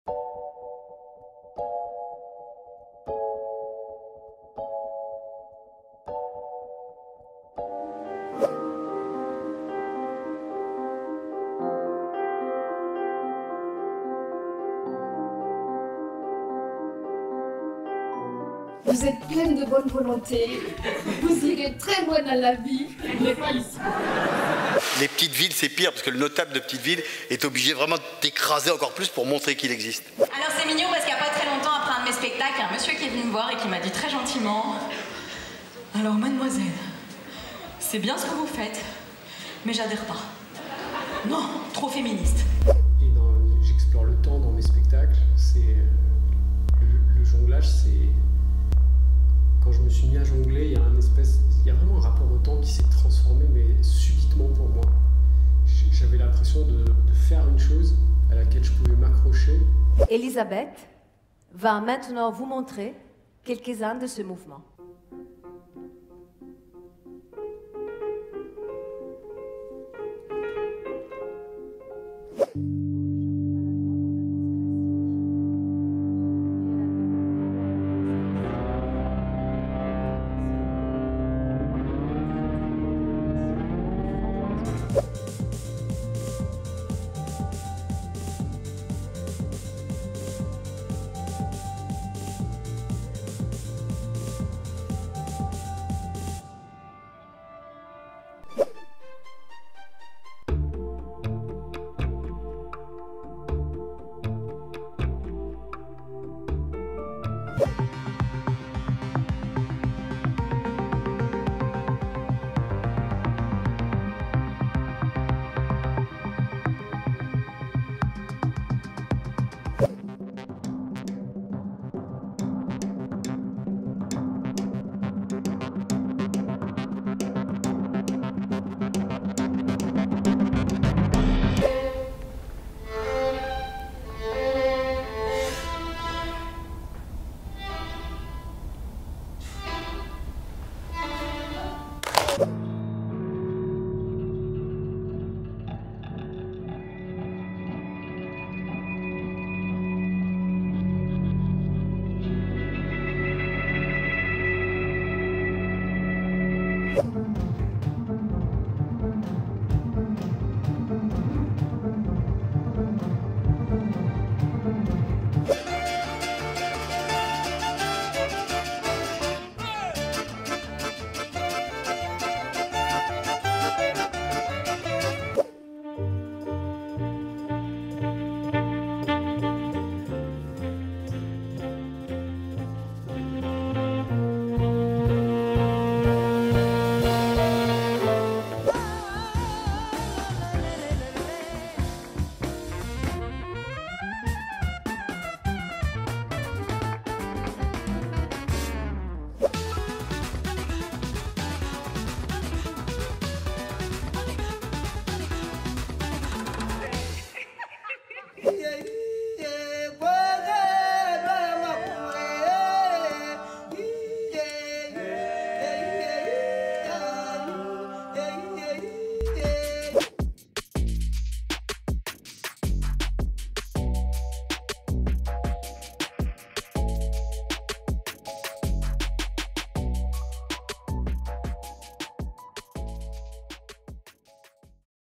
teaser CCM partie 2 / février à juin 2022
Découvrez dès à présent le teaser de la deuxième partie de saison des Centres Culturels de Limoges.
Un avant goût de la programmation de février à juin 2022.
(Un montage réalisé par la 7àLimoges)